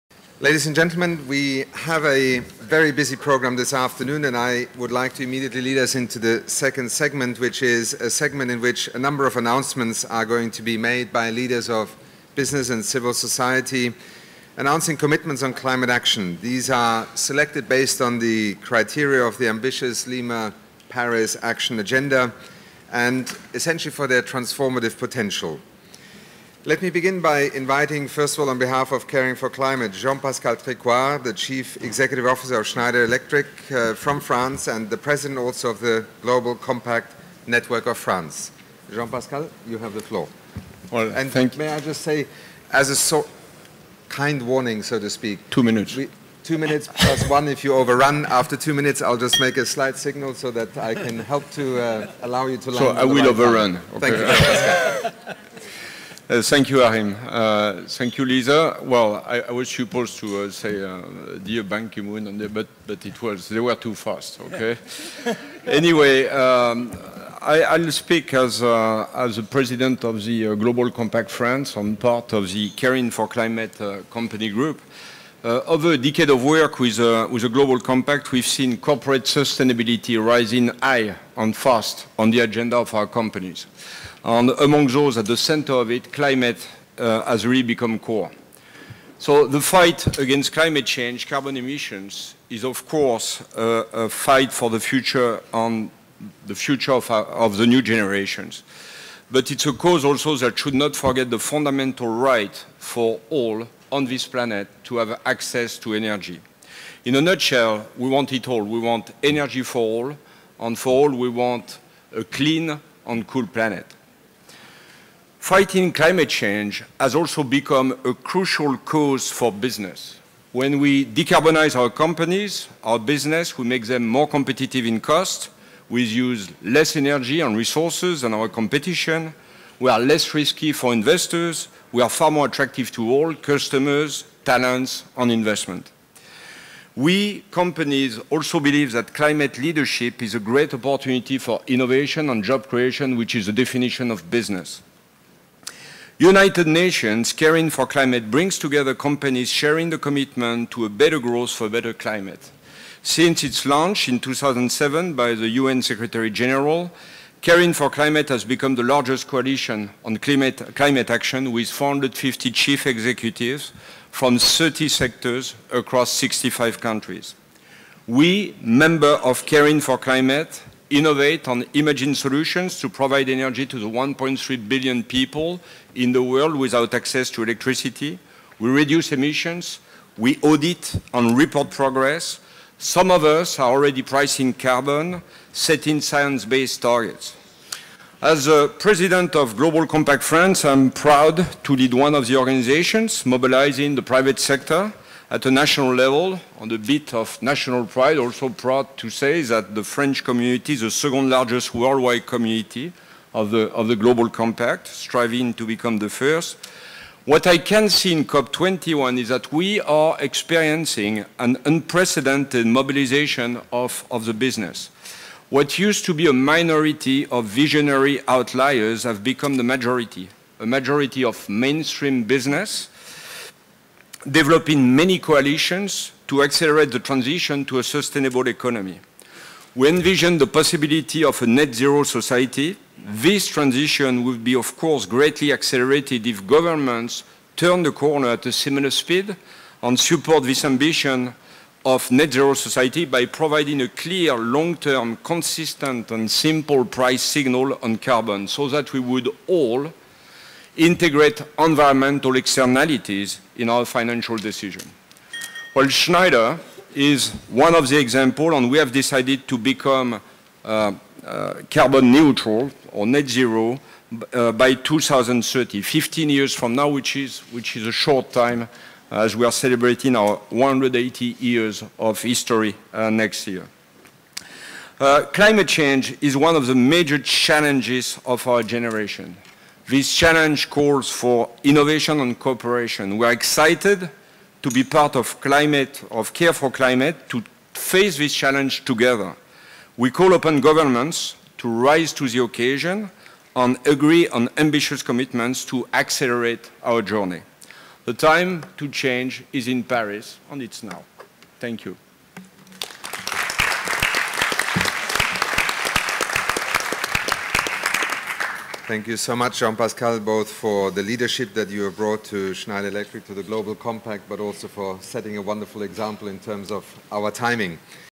C4C Business Forum COP21 – Mr. Jean-Pascal Tricoire, Chairman and CEO, Schneider Electric SE
Chief Executives of Caring for Climate and UNEP-FI signatories engaged in a high-level meeting together with Government representatives, including representatives of relevant bodies established under the UNFCCC. The high-level meeting put forward an overview of all commitments and contributions by business and investors towards COP21. Following welcoming remarks, participants engaged in discussions focused on pricing the cost of carbon emissions. The event concluded with a report-back segment and key recommendations for Government actions that would help bring greater scale and quality to corporate climate leadership globally. The high-level meeting was the focus area business event under the Lima-Paris Action Agenda.

The speaker in this video is Mr. Jean-Pascal Tricoire, Chairman and CEO, Schneider Electric SE.